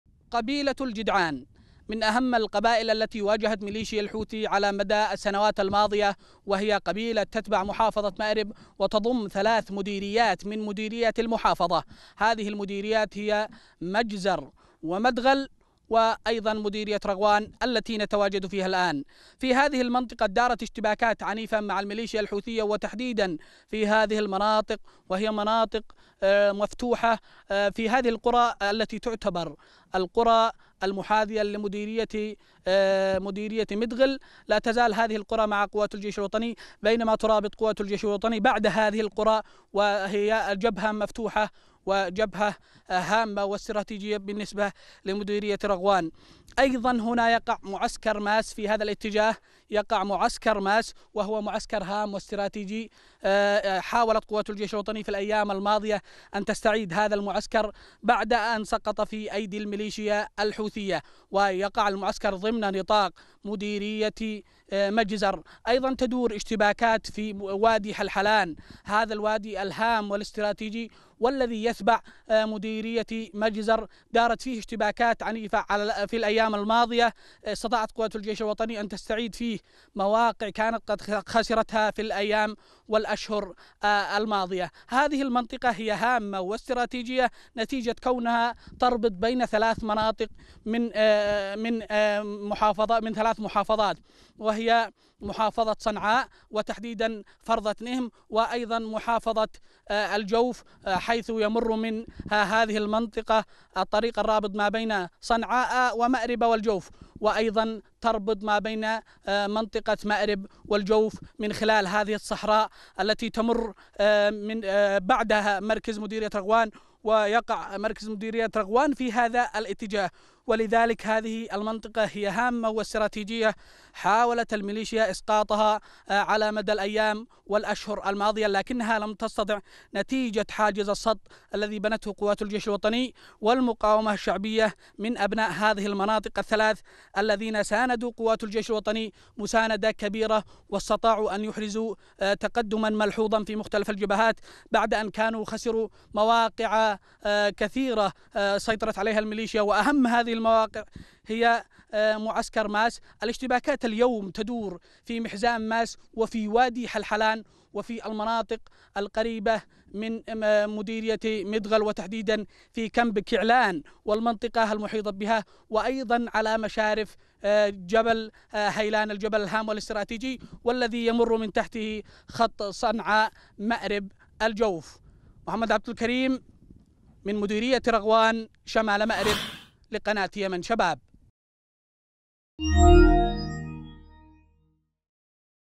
يمن شباب ترصد تطورات المعارك في الجبهة الشمالية الغربية بمأرب
| تقرير محمد عبد الكريم

قناة فضائية يمنية مستقلة تبث على مدار النايلسات بالترددات التالية :
HD     11257/H/27500
SD     11391/V/27500